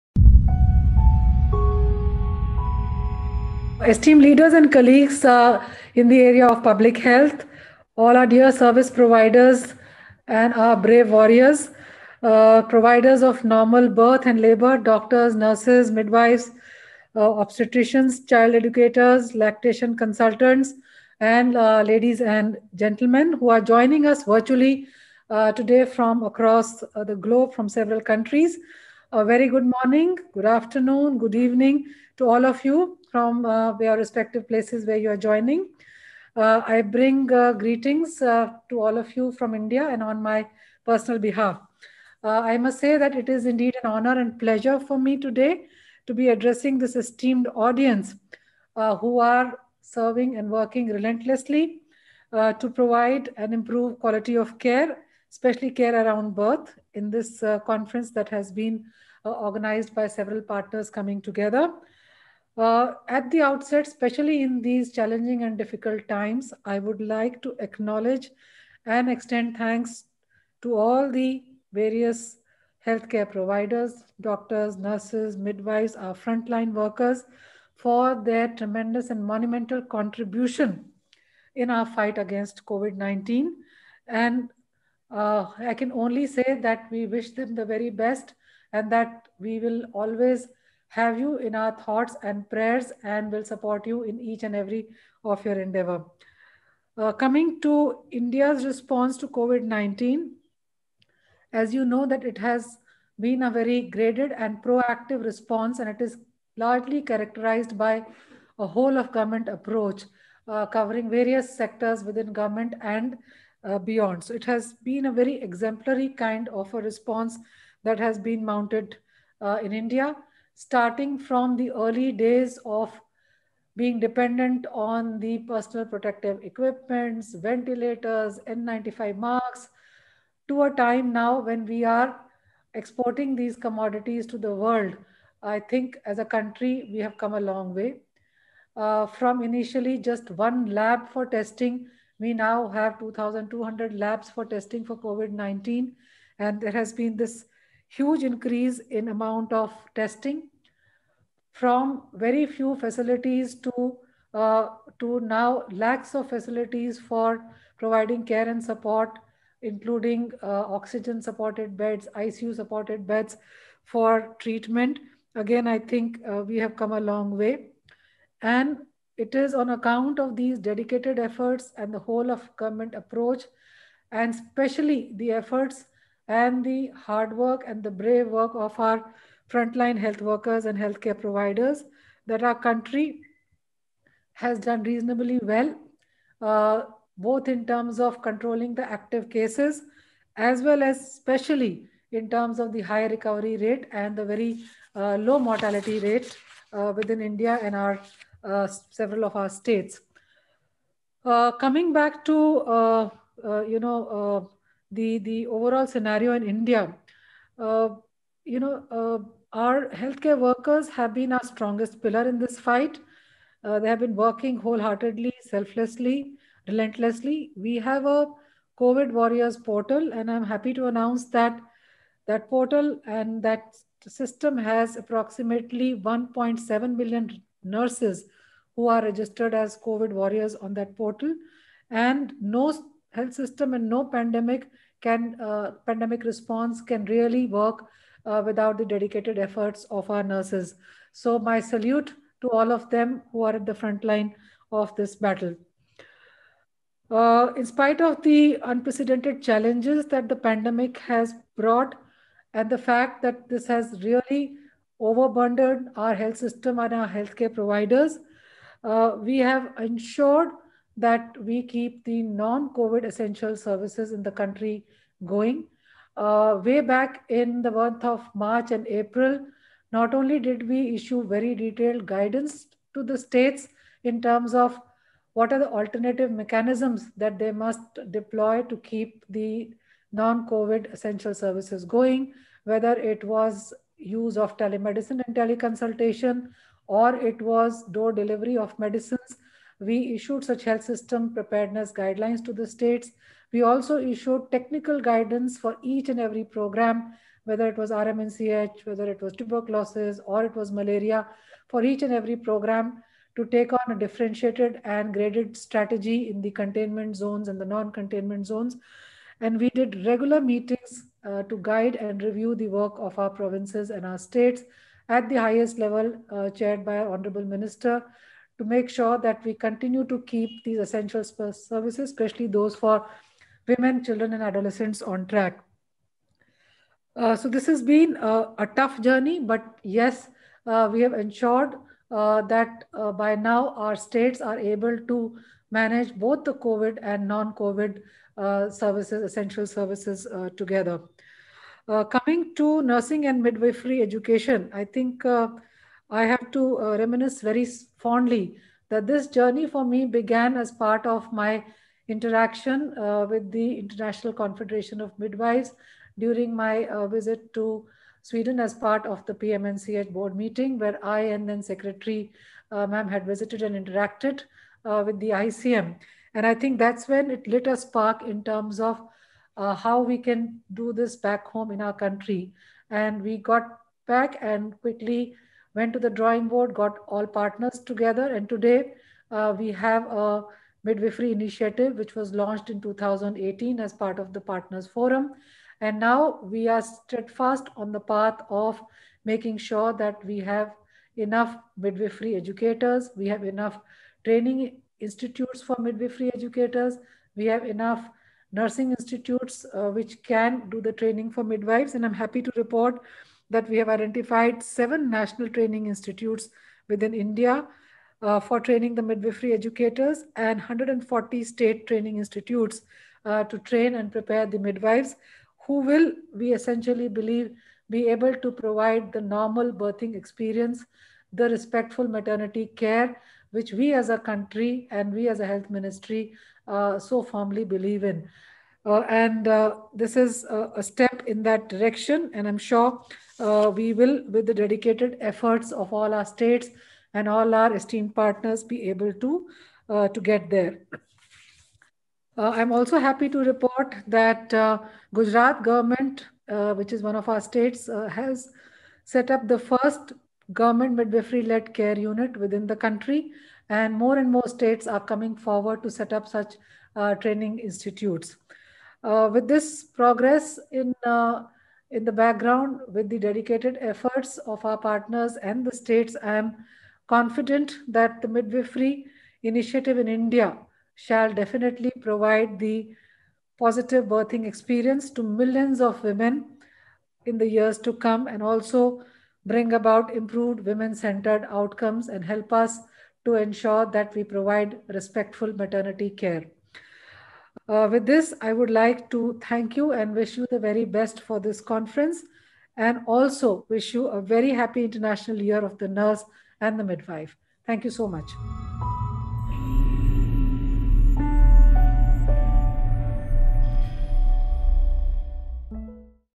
Ms Vandana Gurnani, Addl Secretary and Mission Director, NHM, GoI.
Ms Vandana Gurnani, Addl Secretary and Mission Director, NHM, GoI, talks about the Govt.'s initiatives in introducing 7 national midwifery training institutes in India and 140 state midwifery training institutes across the country.